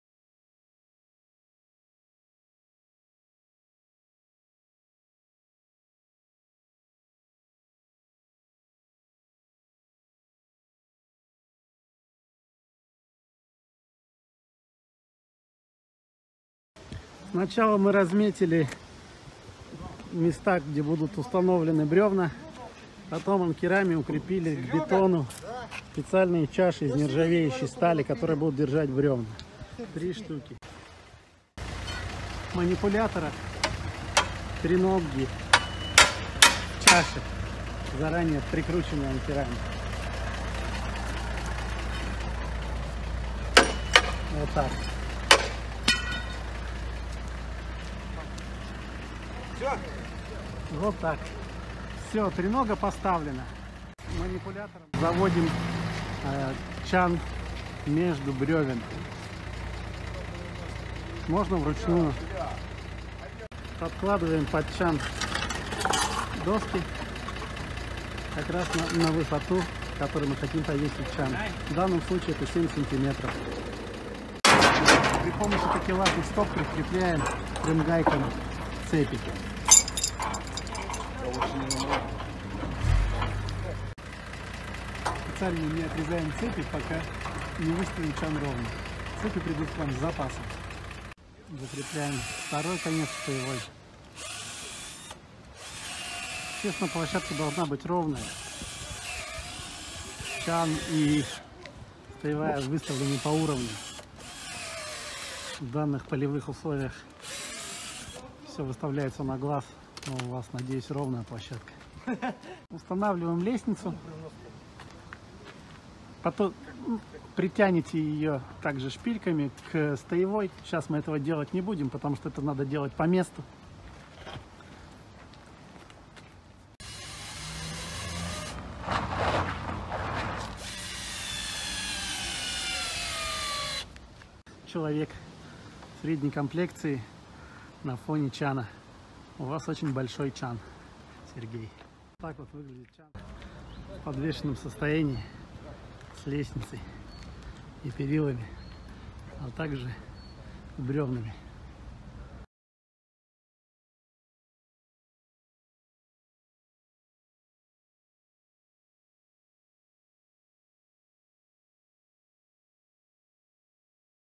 Установка чана на бетонный пол
Руководство по установке Сибирского Банного Чана на бетонное основание.